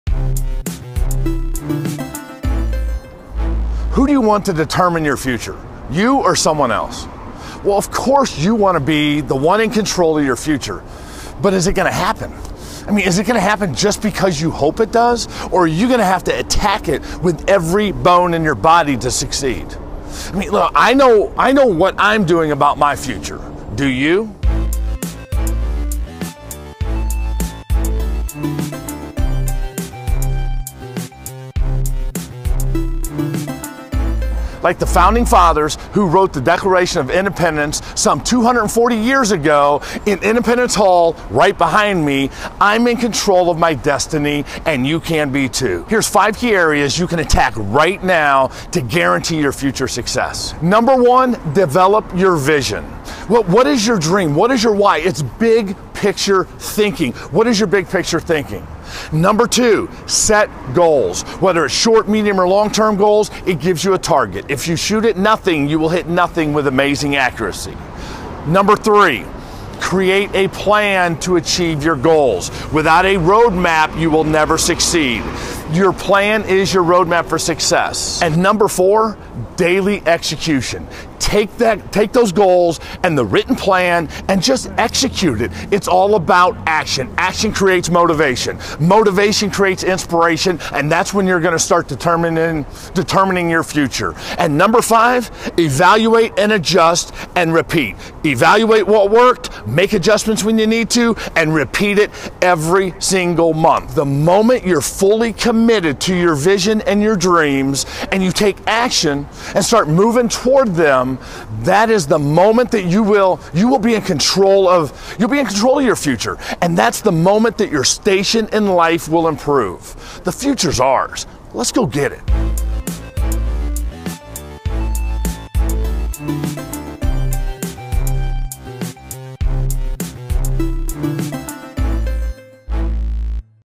Take Control
What are you doing to take control of your sales, to take control of your results and take control of your life? It all starts with action. I know what I'm doing to take control of my future. Do you?

Whether you're selling cars or detailing them, whether you're leading a team as a sales manager or you just want to be on a team, you can be in control of your destiny and create your own future. Here's 5 key areas you can attack right now to guarantee your future success.

timkintzcartraining